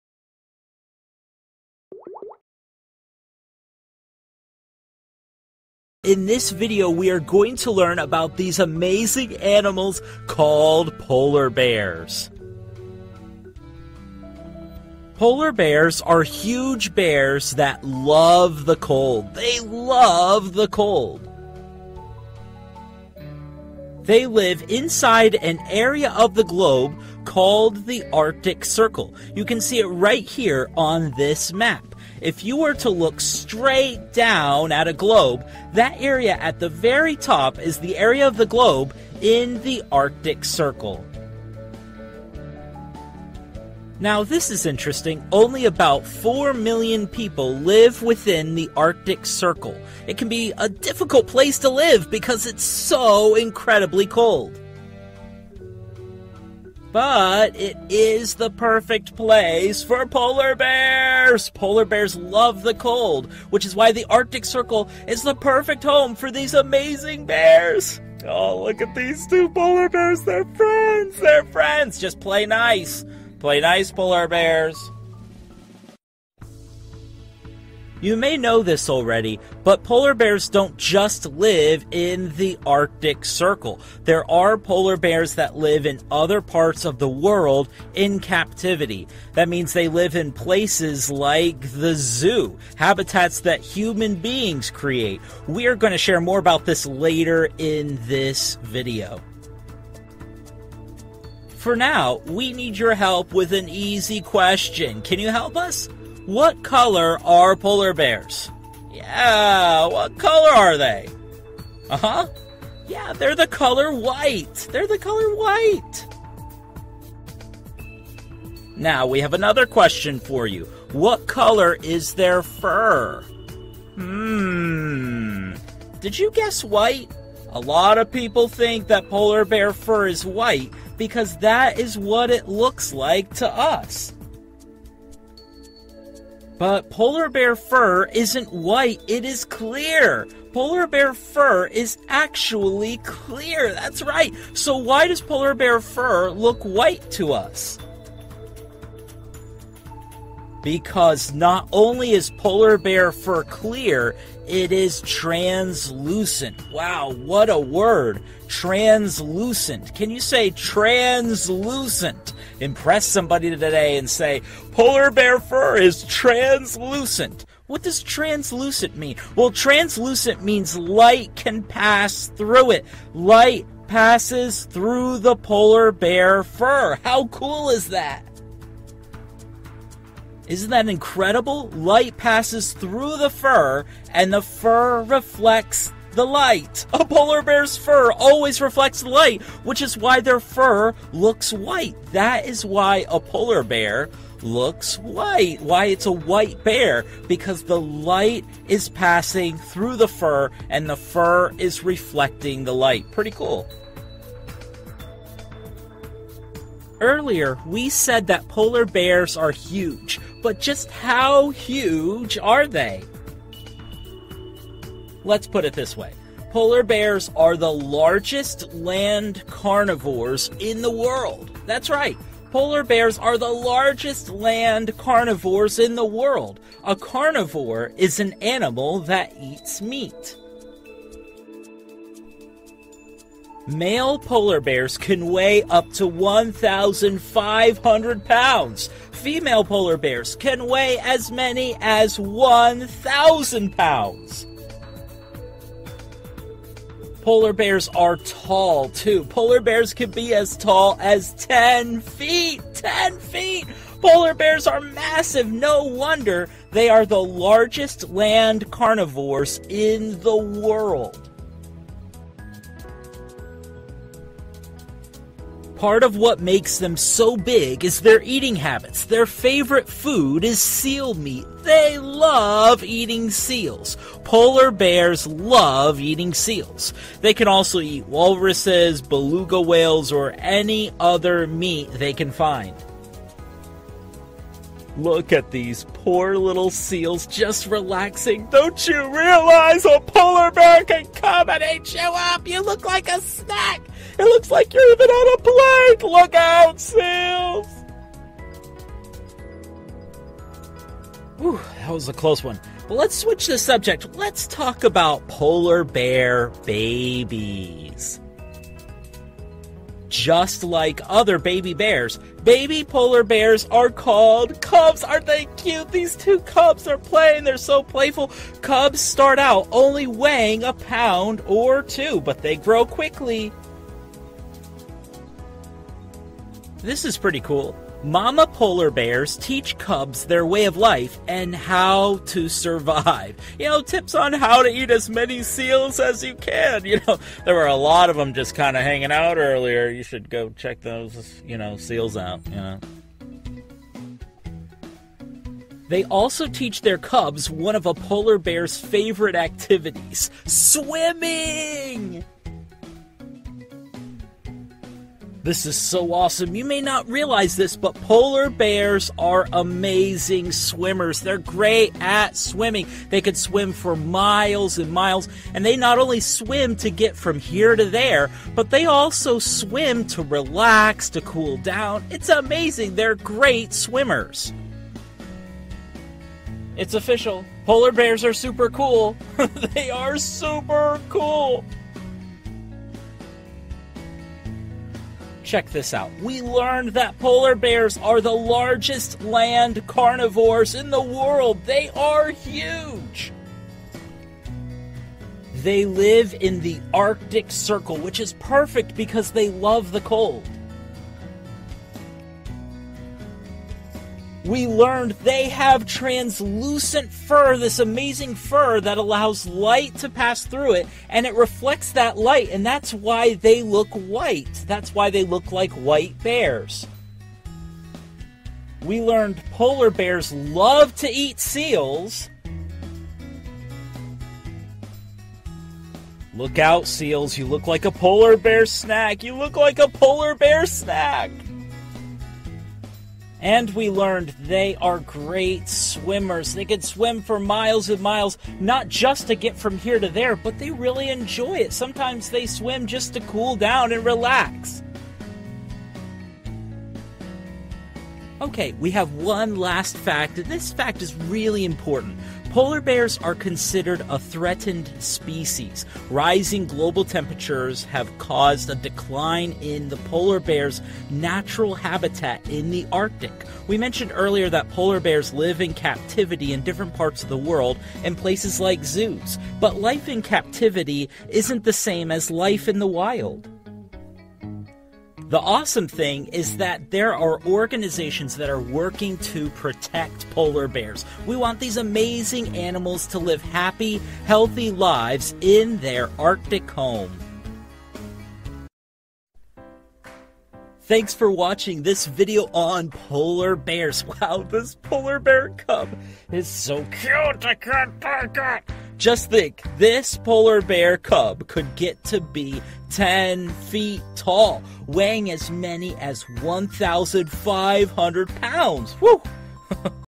Polar Bear Facts for Kids
Fun facts about polar bears! This polar bear learning video for kids is the classroom edition of our polar bears video. The classroom edition videos feature the learning content without the kids comments. Which polar bear fact was your favorite?

00:00 What are polar bears?
0:22 Where do polar bears live?
1:41 What color are polar bears?
3:25 How big are polar bears?
4:24 What do polar bears eat?
5:43 Polar bears are swimmers
6:22 Review